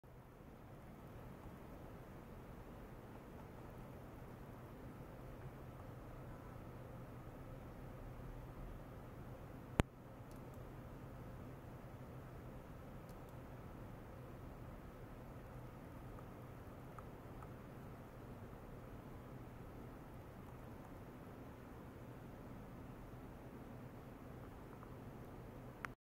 ポリイミドフィルムに鉄めっきを行った部材を利用した超音波プローブによるメガヘルツ超音波のダイナミック制御実験（超音波システム研究所）
５００Ｈｚから９００ＭＨｚの超音波伝搬状態を制御可能にする

非線形共振型の超音波発振条件を利用することで

　２０Ｗ以下の超音波出力で、５０００リッターの水槽でも、

参考